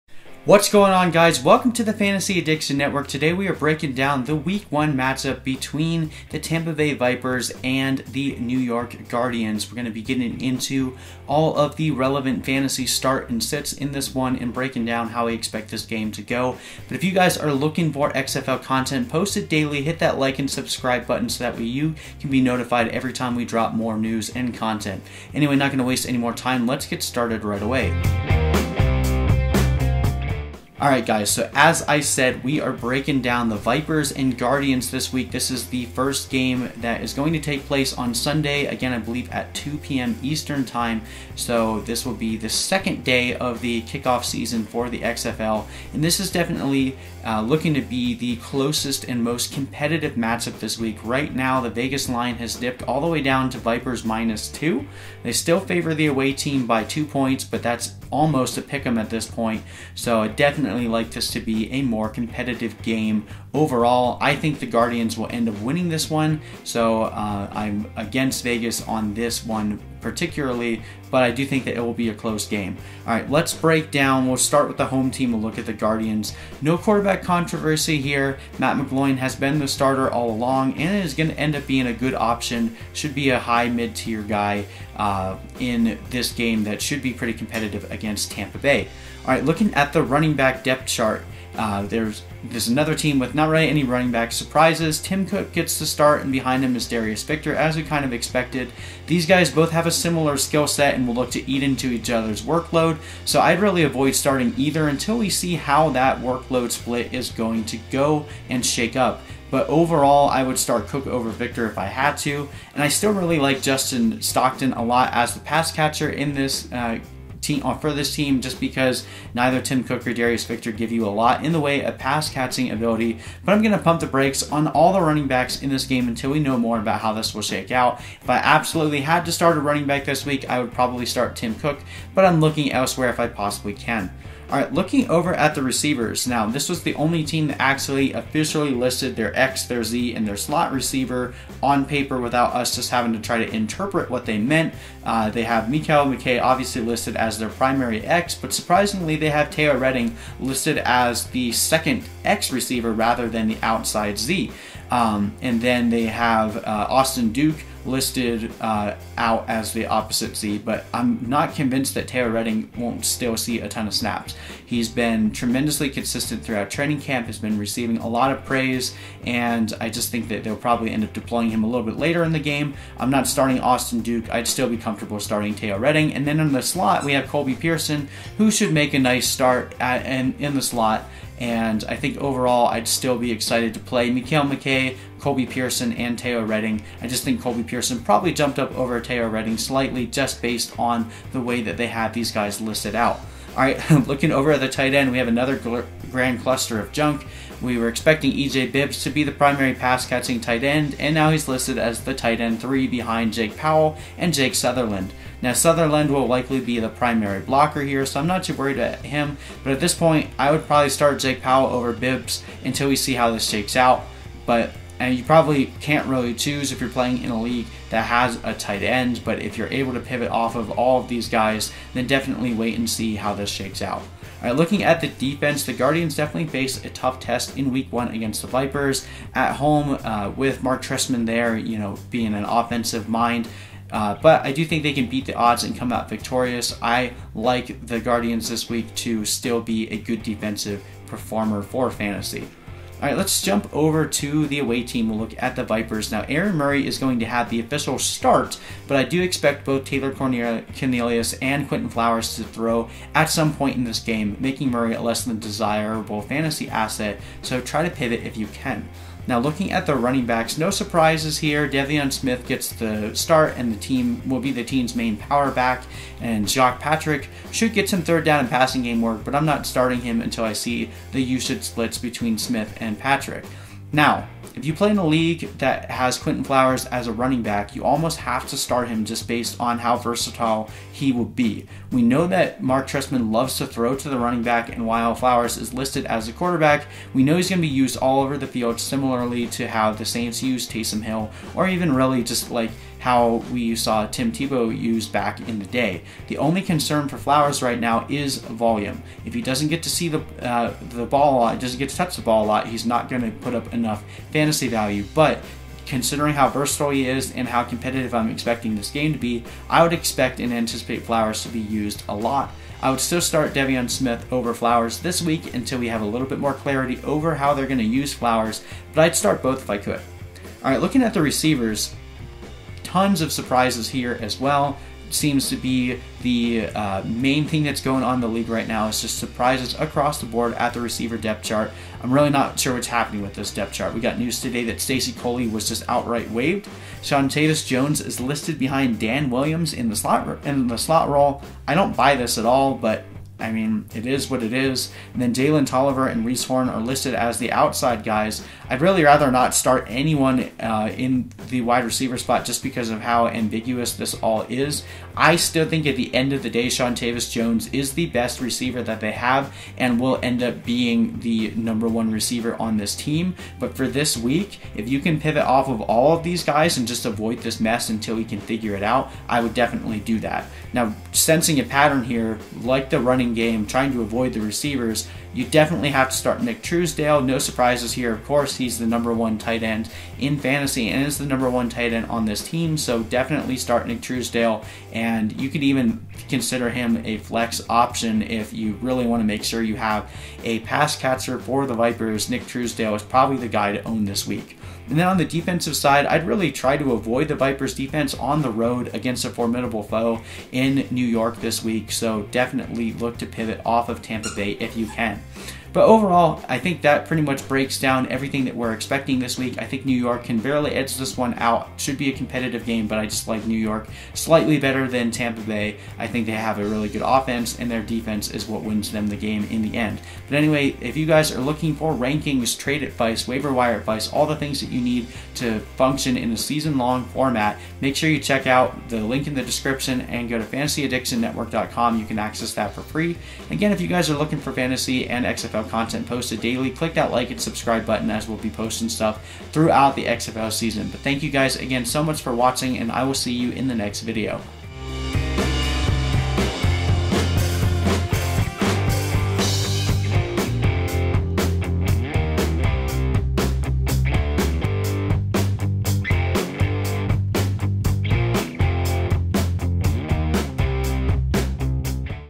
XFL Fantasy Football Start Sit Vipers vs. Guardians | XFL GAME PREVIEW
Hey guys, we are ONLY 2 DAYS FROM THE START OF THE XFL SEASON! In this video we are previewing the Tampa Bay Vipers vs. New York Guardians and going over all the fantasy relevant players to help you with those pivotal start sit decisions!

If you’re short on time, here’s a condensed version of my starts/sits outlined in this video.

These are the players I would recommend starting this week:
Matt McGloin
De'Veon Smith
Quinton Flowers
Mekale McKay
Colby Pearson
Teo Redding
Nick Truesdell
Jake Powell
Guardians DEF

These are the players I would take a second guess on and possibly avoid this week:
Aaron Murray
Tim Cook
Justin Stockton
Jacques Patrick
Seantavius Jones
Reece Horn
Jalen Tolliver
Austin Duke
EJ Bibbs
Vipers DEF

These are the players I would recommend sitting this week:
Darius Victor
Donteea Dye